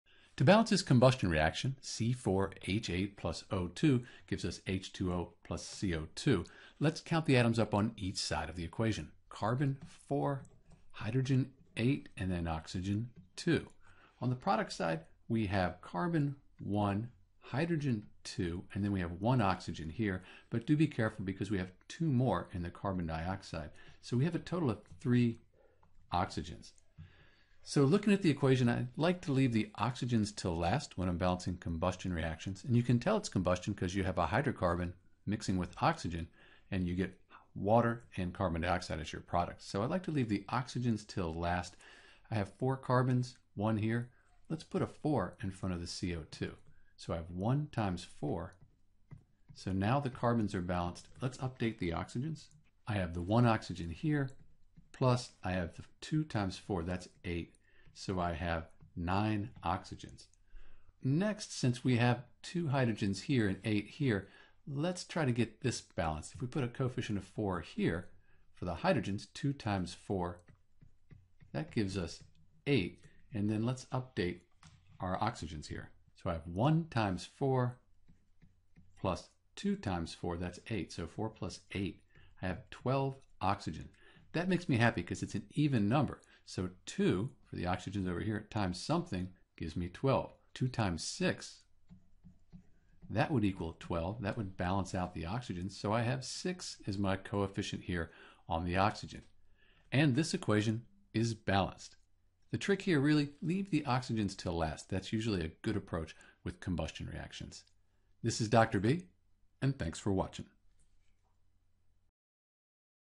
How to Balance C4H8 + O2 = H2O + CO2
In this video we'll balance the equation C4H8 + O2 = H2O + CO2 and provide the correct coefficients for each compound.

To balance C4H8 + O2 = H2O + CO2 you'll need to be sure to count all of atoms on each side of the chemical equation.

Once you know how many of each type of atom you can only change the coefficients (the numbers in front of atoms or compounds) to balance the equation for Hydrocarbon + Oxygen gas.